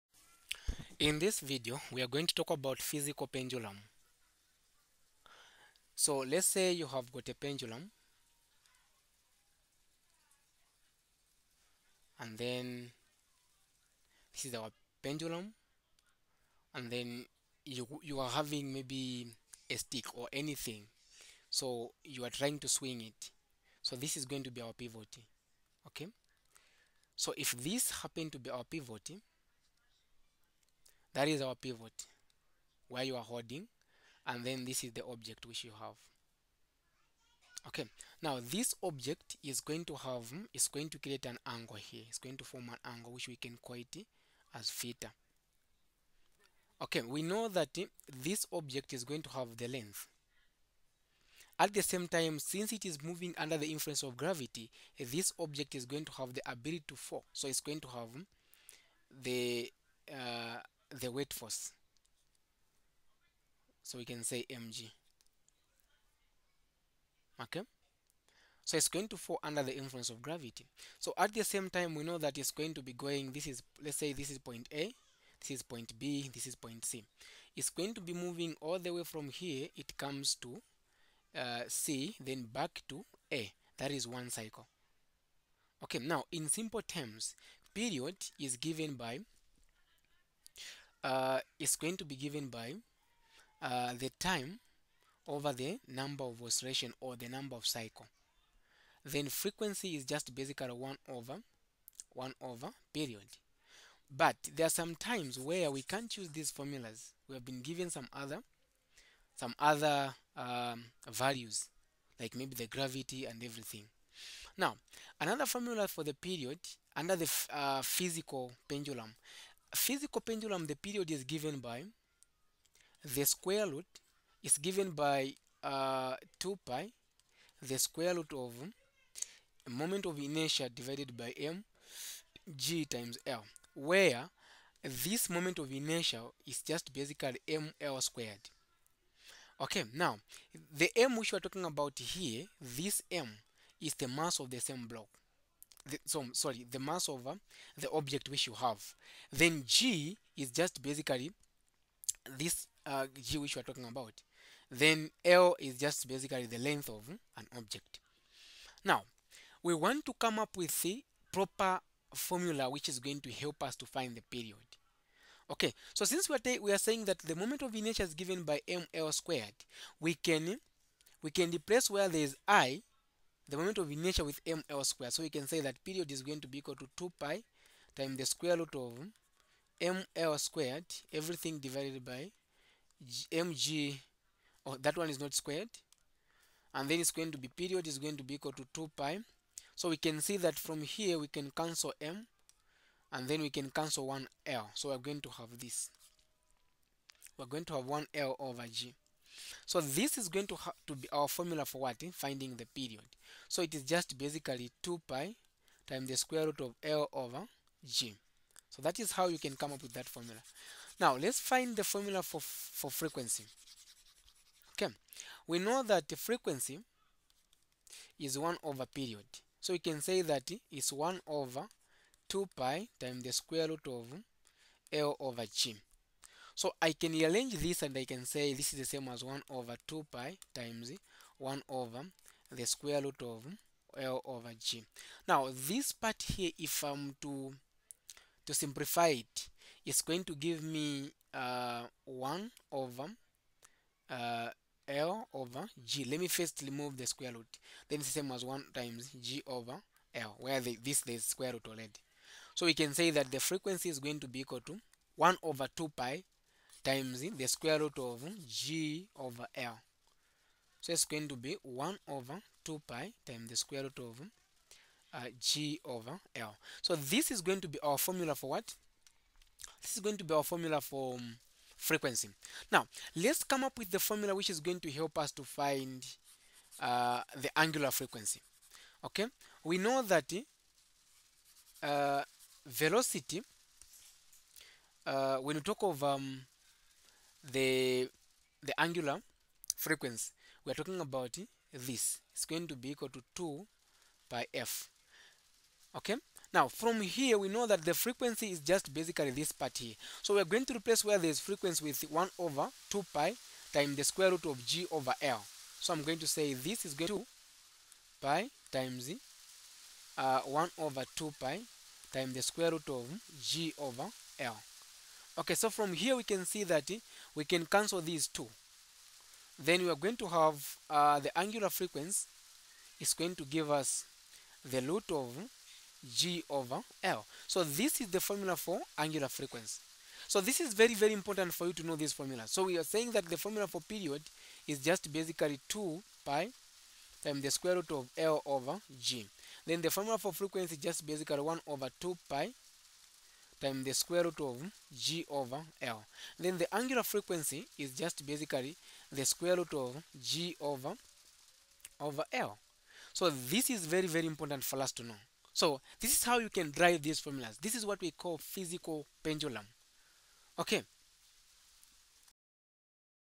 physical pendulum-Period and Frequency
In this video, we're going to look at the physical pendulum- Period and Frequency. First, we'll discuss the physical pendulum-period relationship, and then we'll look at the physical pendulum-frequency relationship.

This video is a great way to learn about the physical pendulum- Period and Frequency, and to understand how they can be used in physics. Be sure to check it out!